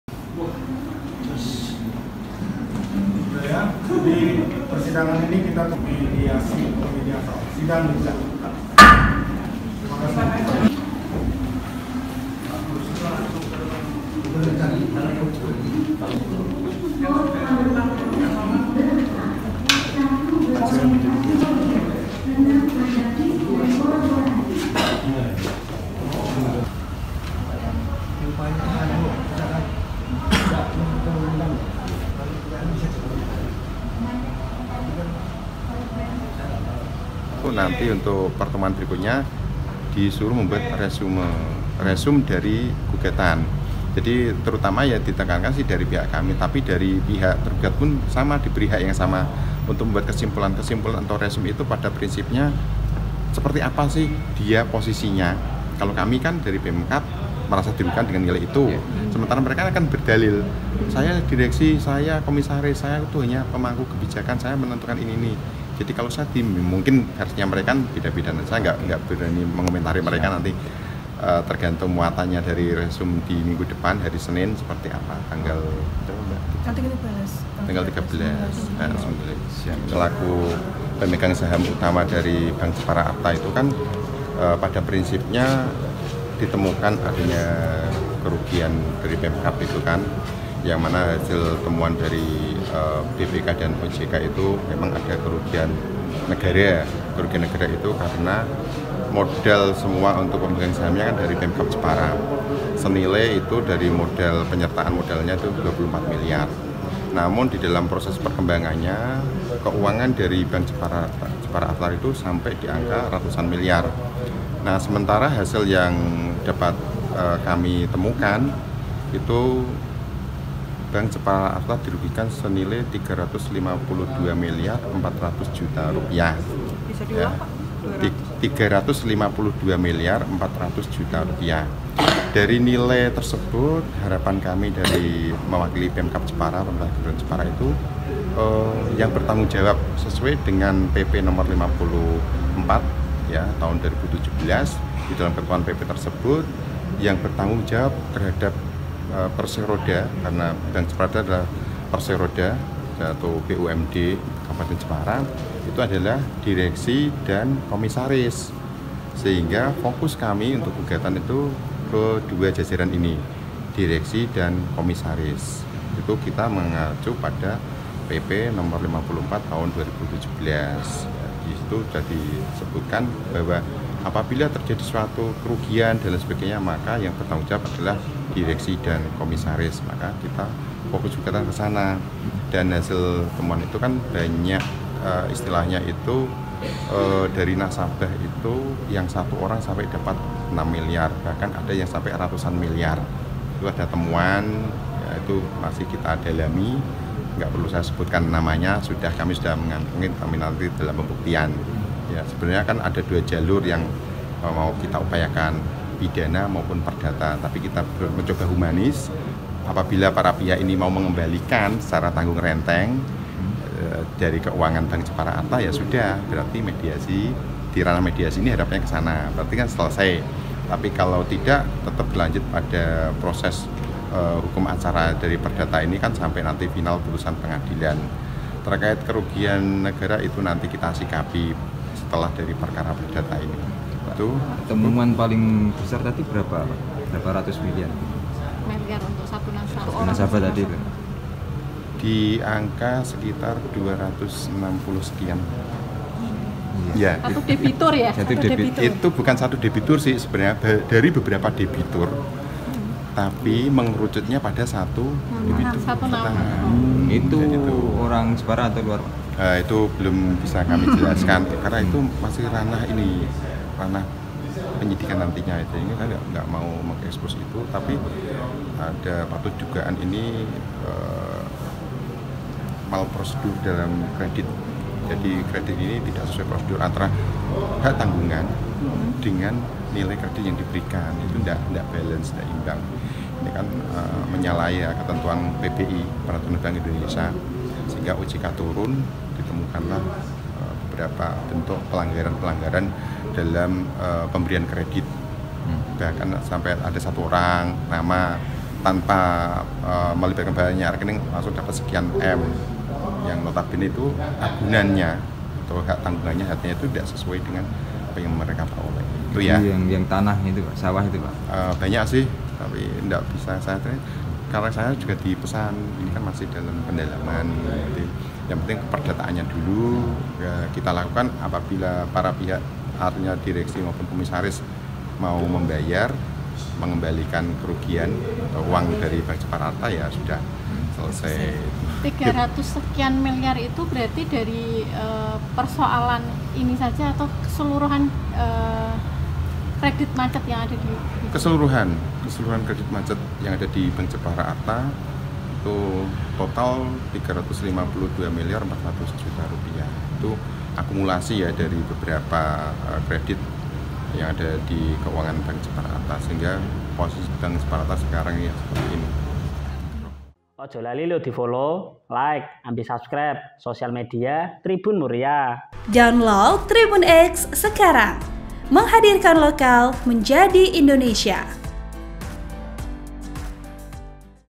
Sidang Kasus BJA Dimulai Dengan Mediasi, Pemkab Jepara Dirugikan Rp 24 Miliar
Sidang kasus PT BPR Bank Jepara Artha (BJA) dimulai dengan tahapan mediasi dari kuasa hukum Pemkab dengan BJA.

Dari mediasi pertama, Tim Kuasa Hukum Pemkab Jepara, Mursito mengatakan bahwa Pemkab Jepara meminta pertangungjawaban atas modal awal milik Pemerintah Rp 24 Miliar di BJA.

Namun seiring waktu, permasalahan BJA semakin besar hingga ditemukan adanya kerugian sampai ratusan miliar.

Tim Kuasa Hukum Pemkab Jepara sebenarnya akan mengambil dua jalur tindakan hukum, mulai dari pidana maupun perdata.

Tim Kuasa Hukum Pemkab Jepara memilih untuk menempuh jalur mediasi terlebih dahulu sembari menunggu iktikad baik dari pihak BJA.

Editor: edr